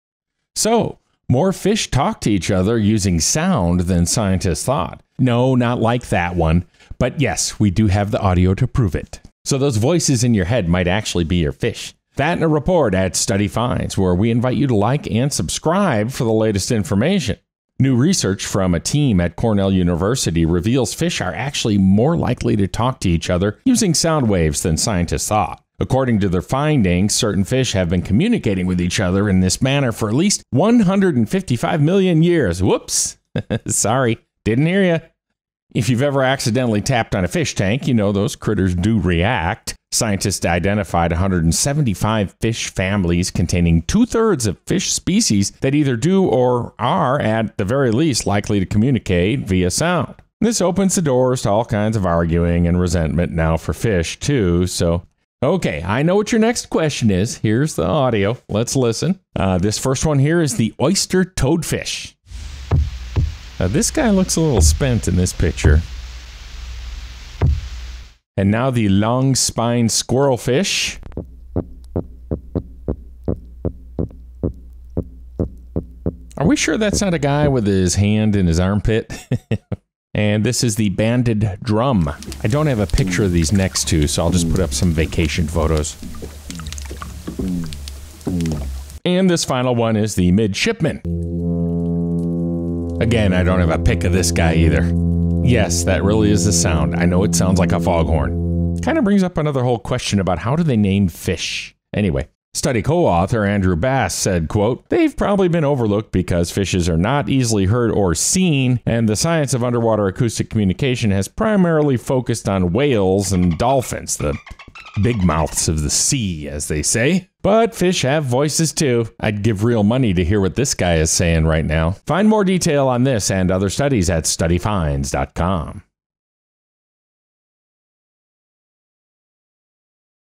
Here's What Fish Sound Like When They 'Talk' To Each Other
Those voices in your head might actually be your fish! More fish ‘talk’ to each other using sound than scientists thought! And we have the audio to prove it!

New research from a team at Cornell University reveals fish are actually much more likely to “talk” to each other using sound waves than scientists thought.

According to their findings, certain fish have been communicating with each other in this manner for at least 155 million years! Whoops... sorry... didn't hear you!

If you've ever accidentally tapped on a fish tank, you know those critters react.

Scientists identified 175 “fish families” containing two-thirds of fish species that either do, or are at the very least likely to, communicate via sounds.

Which opens a door to all kinds of arguing and resentment now for fish too!

Ok... I know what your next question is... here is the audio...

Here is the Oyster Toadfish

Now the Longspine squirrelfish,

The Banded drum

This is the Midshipman

Study co-author Andrew Bass said – QUOTE - “They’ve probably been overlooked because fishes are not easily heard or seen, and the science of underwater acoustic communication has primarily focused on whales and dolphins. But fishes have voices, too!”

FIND MORE DETAIL on this and other studies at AT STUDYFINDS.COM

Video Credit:
Videvo – Videvo.net
Pexels.com

Audio
Oyster Toadfish, William Tavolga, Macaulay Library
Longspine squirrelfish, Howard Winn, Macaulay Library
Banded drum, Donald Batz, Macaulay Library
Midshipman, Andrew Bass, Macaulay Library

Image Credit:
Aaron Rice.
Jeffrey T. Williams
Texas Parks And Wildlife